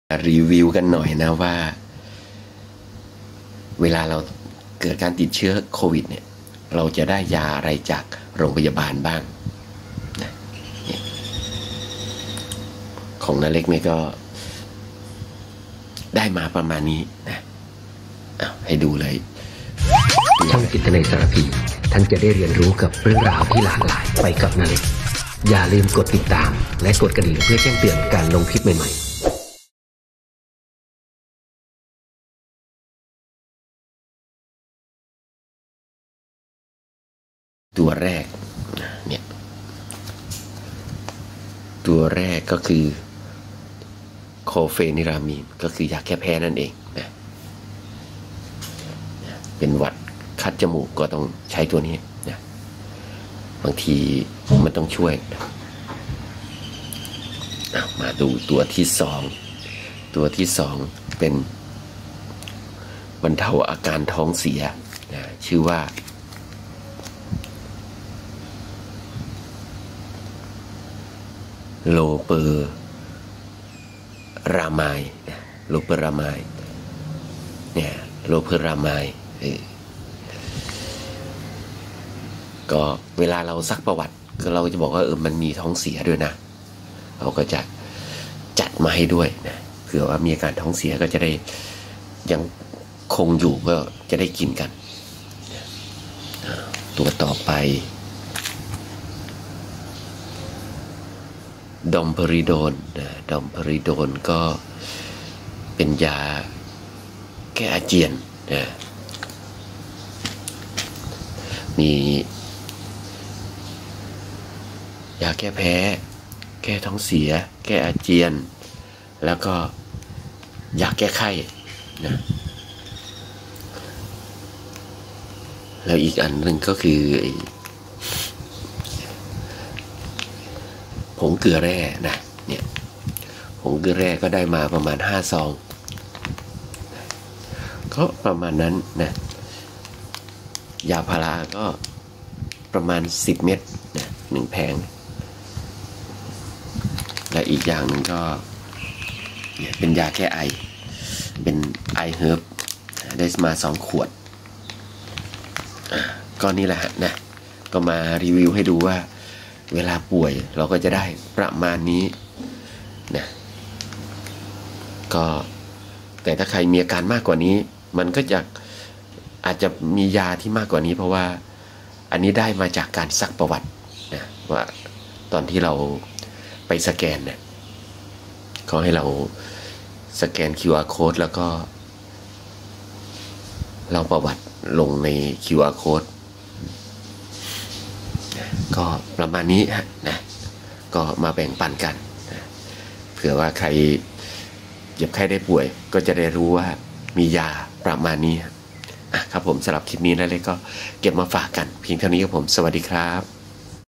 น้าเล็กรีวิว#: ถ้าเราติดเชื้อโค...ยอดฮิต ยาที่ได้จากโรงพยาบาลมียาอะไรบ้าง...?
# น้าเล็กรีวิว